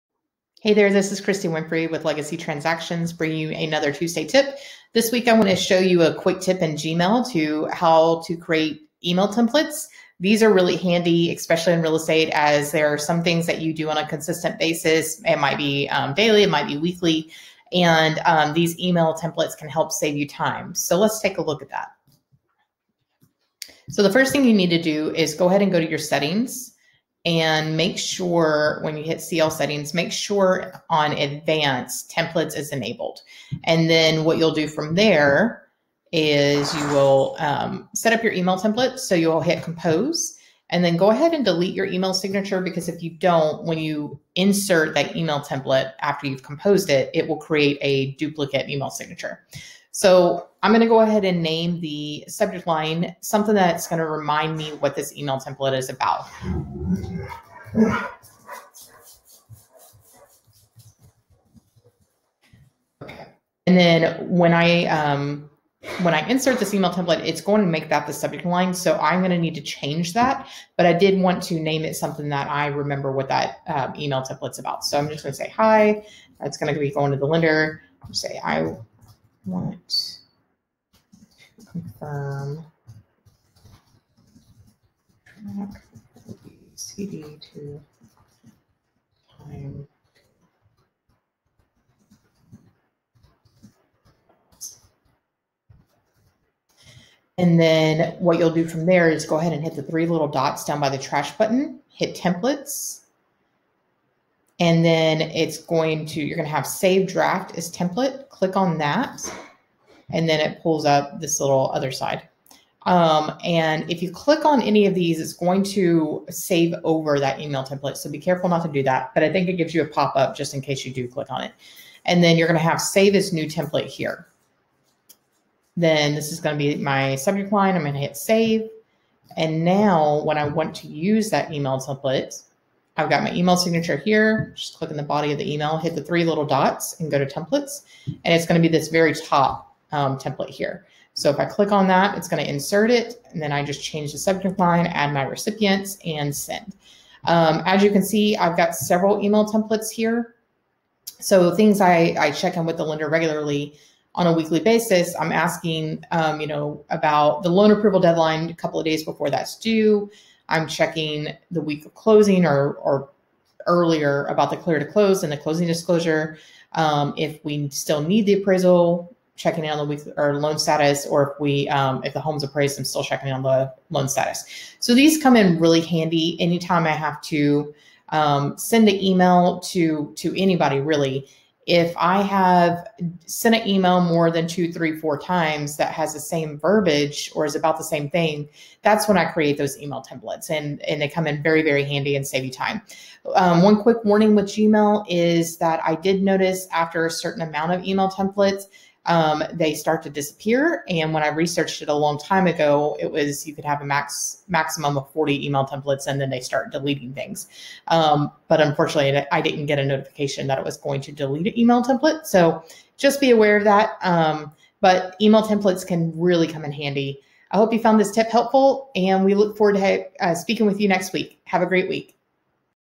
How to create Gmail email templates to save time
Here is our newest Tuesday Tip. Creating Gmail email templates to save time